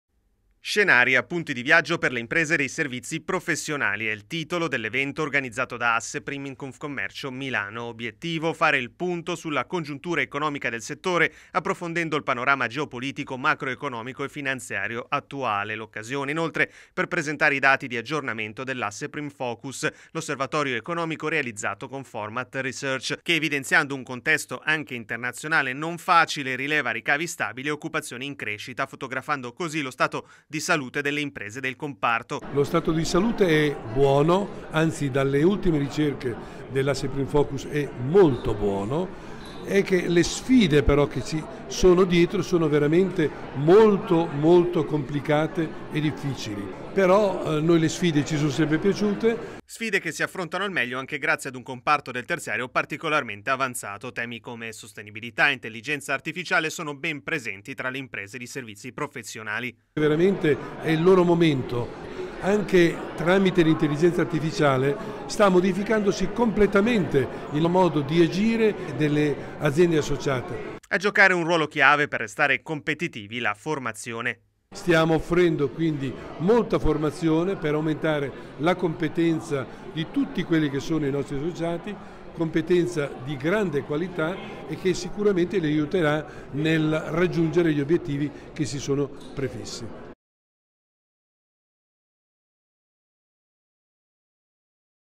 Asseprim, “Scenari: appunti di viaggio per le imprese di servizi professionali”
L’evento organizzato da Asseprim in Confcommercio Milano. Un’occasione per fare il punto sulla congiuntura economica del mondo dei servizi professionali, approfondendo il panorama geopolitico, macroeconomico e finanziario attuale.